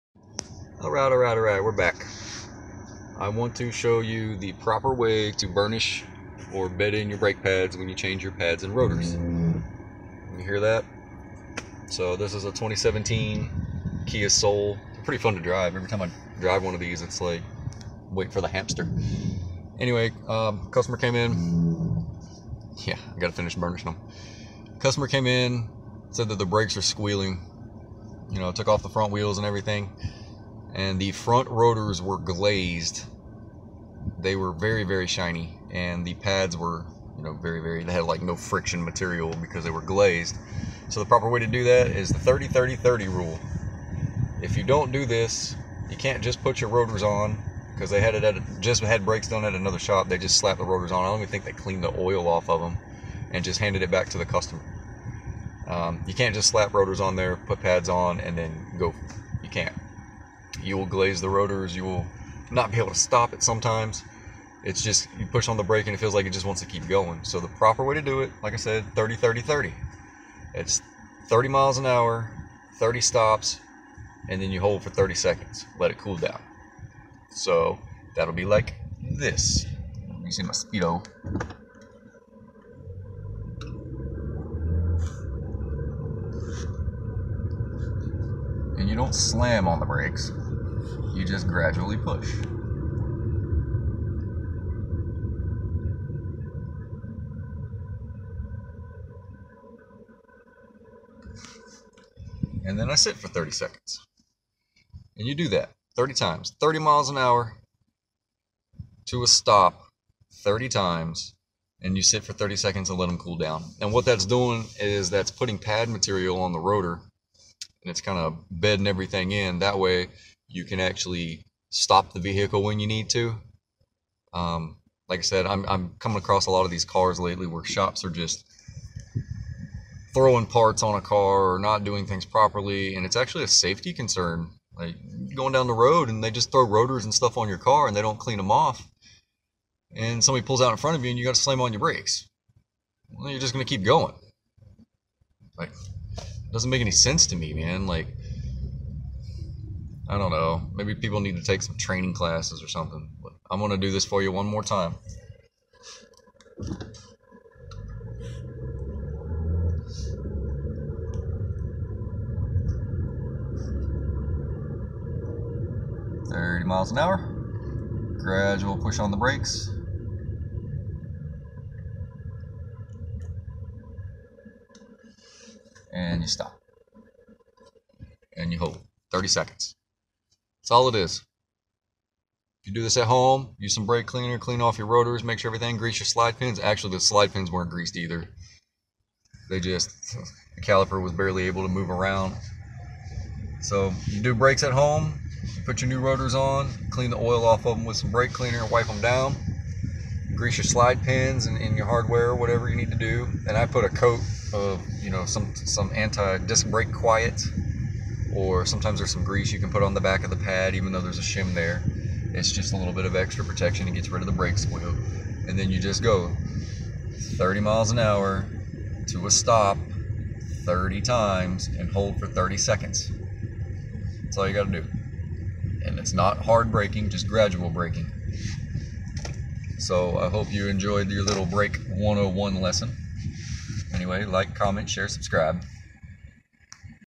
The Proper Way To Burnish/Bed In New Brake Pads.
2017 Kia soul. Customer complaint. Brake squeal.  cause. Front rotors and pads were glazed and slide pins not greased due to another shops lack of knowledge on brakes.

After install. clean the rotors with brake clean. grease the slide pins. You can go the extra step and spray disc brake quiet on back of pad(not pad material) or brake grease.

Go do the burnish process. 30/30/30 rule. 30mph 30 stops 30 sec rest.  This puts pad material on rotors and puts them through a heat cycle.

If not done then you will glaze your new rotors and pads and have the possibility of brake fade and not be able to stop.

Hope you enjoyed brake rotors and pads 101